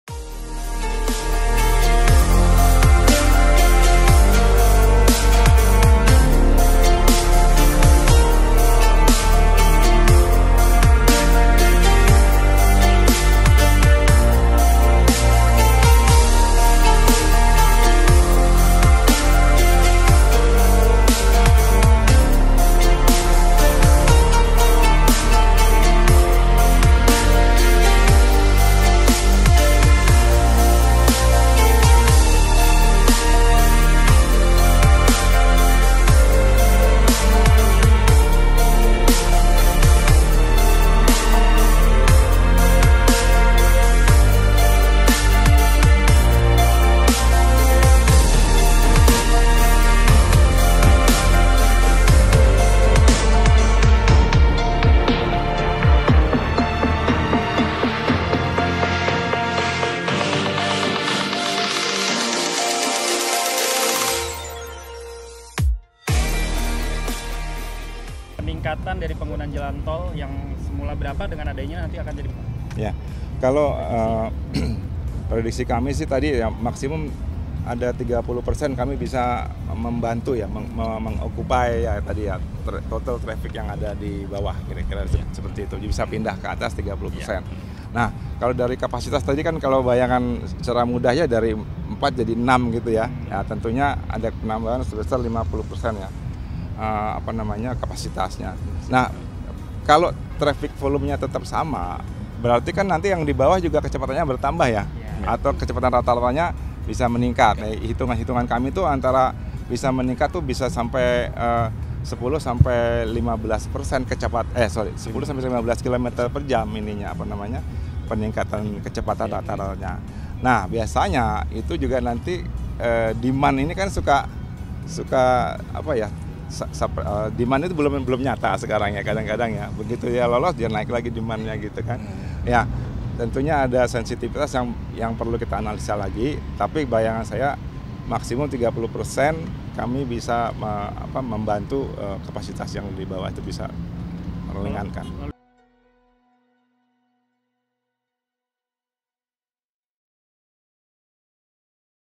Jalan Tol Layang Japek II Dapat Kurangi Kemacetan?
Pengoperasian jalan tol layang diharap mengurangi kemacetan di Jalan Tol Jakarta-Cikampek (Japek) hingga 30 persen. Direktur Utama PT Jasamarga Jalanlayang Cikampek (JJC), Djoko Dwijono, mengatakan jalan layang tersebut bisa mengurangi beban jalan di bawahnya yang dipenuhi kendaraan.⁣⁣⁣
Rencananya, Presiden Joko Widodo akan meresmikan Tol Jakarta-Cikampek (Japek) II Elevated atau Tol Layang Japek hari ini, Kamis (12/12/2019). Jalan sepanjang 36,4 Kilometer itu akan dibuka untuk umum setelah pembersihan dan rambu jalan lengkap.